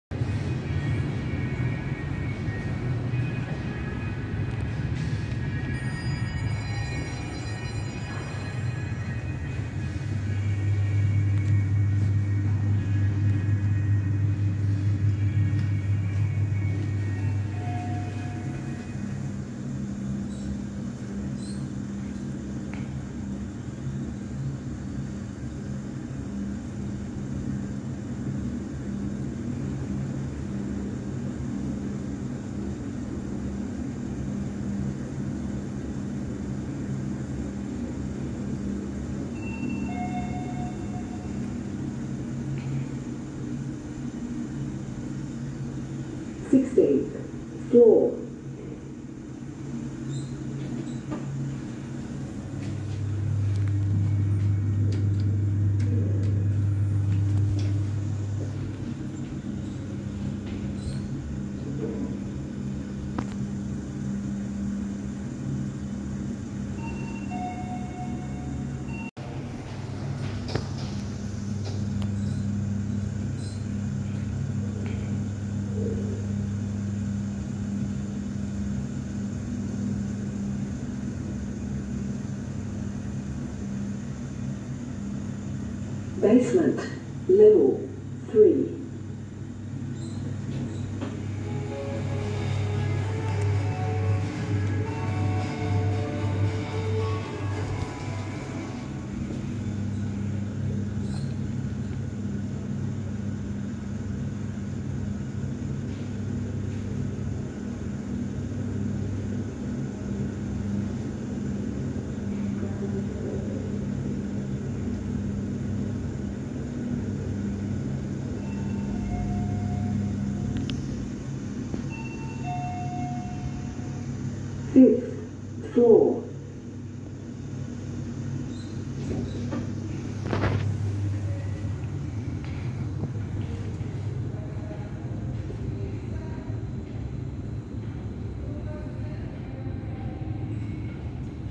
TKE thyssenkrupp Passenger Lift at Sunway Velocity Hotel, Cheras
Pretty fast lift for a short distance. I've been in lifts that serve taller buildings that go slower than this.

Floors served (unknown keycard requirements): G, 5, 7 - 16
Some lifts only: B3, B2